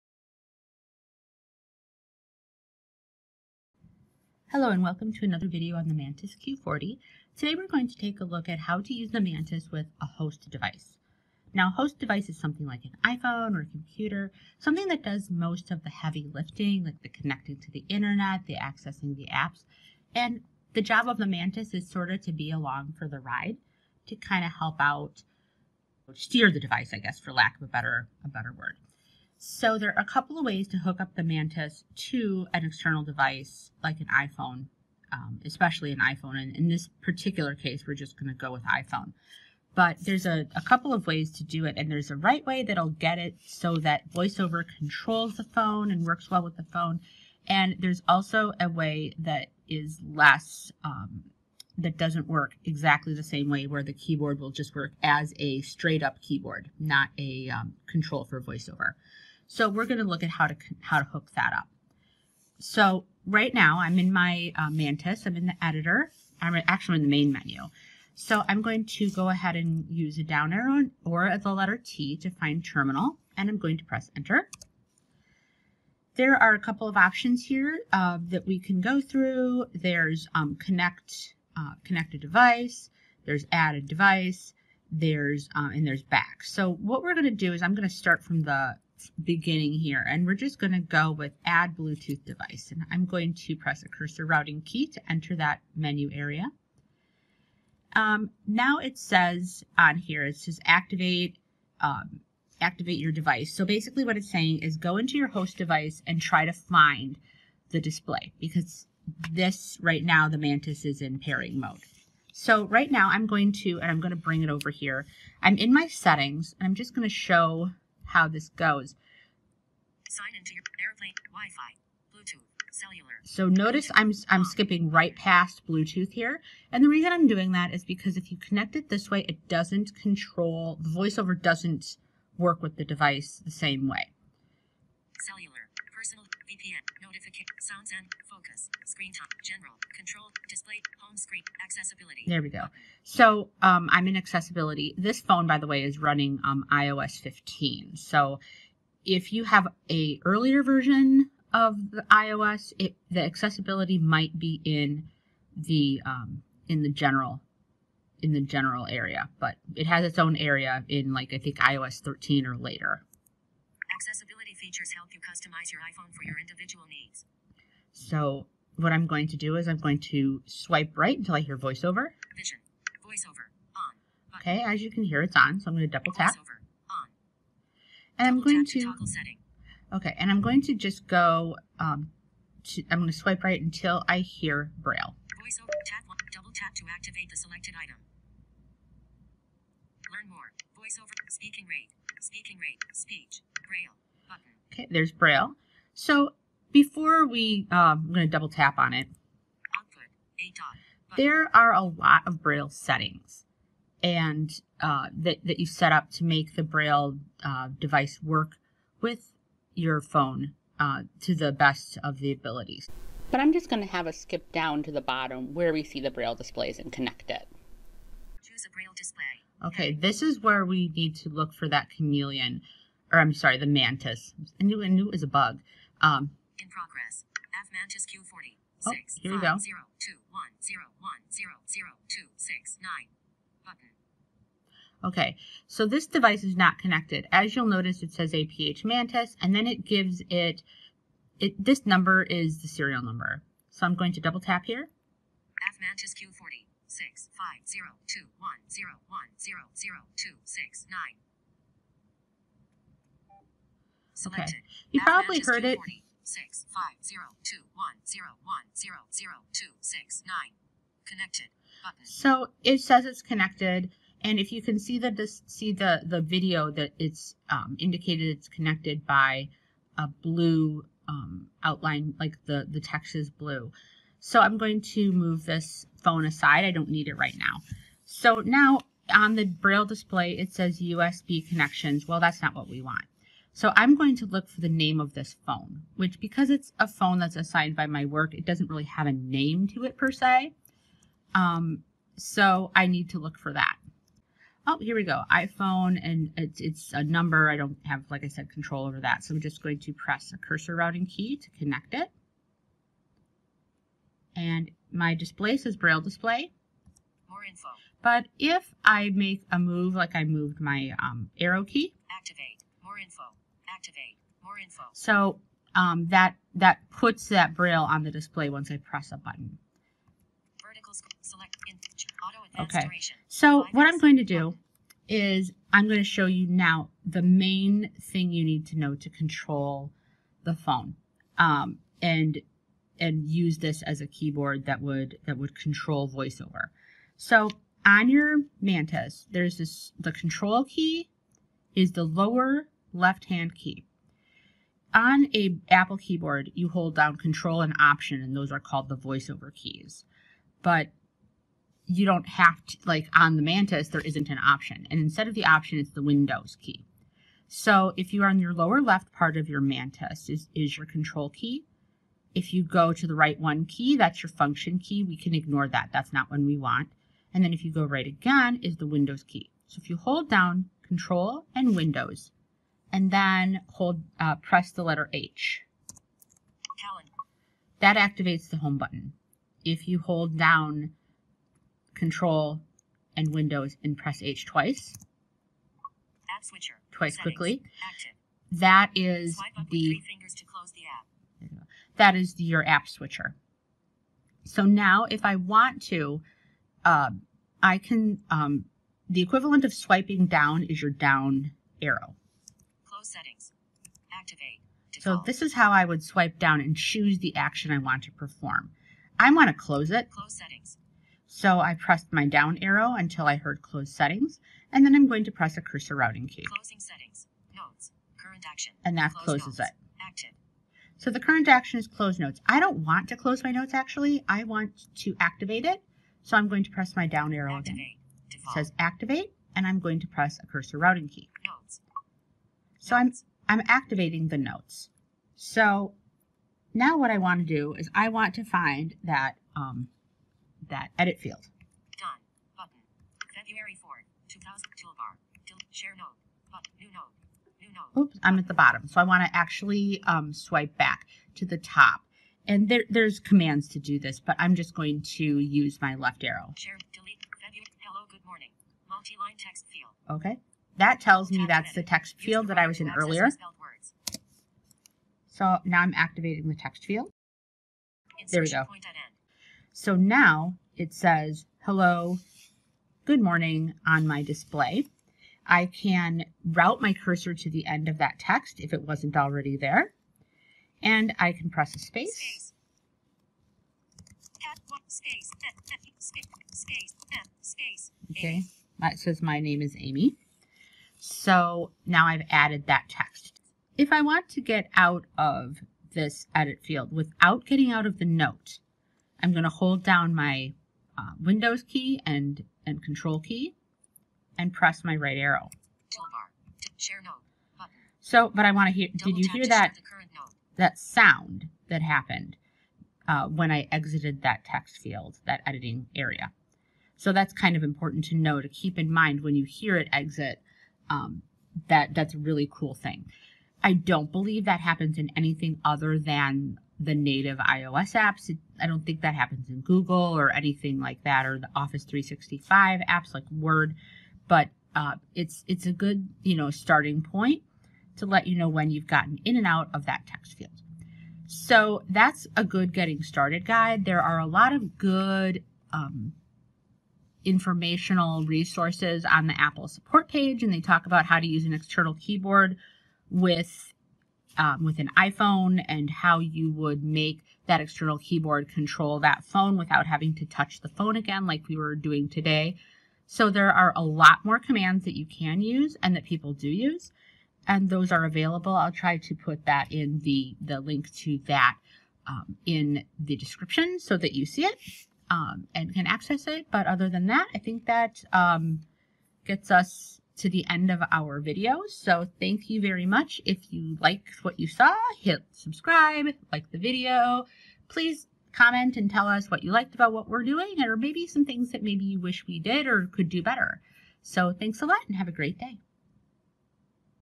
Mantis Part 13: Connect an iOS Device
This video covers connecting the Mantis to an iOS device.